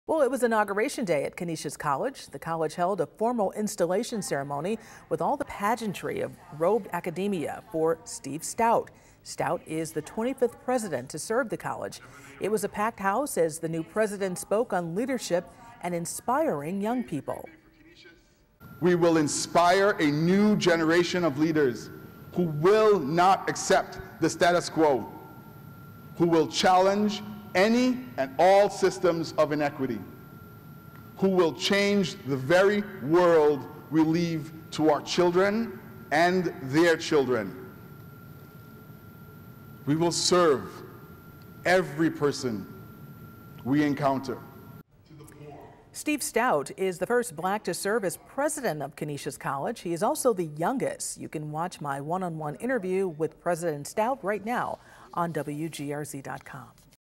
Inauguration day at Canisius College
Steve Stoute was made the college's 25th president at a ceremony on Saturday.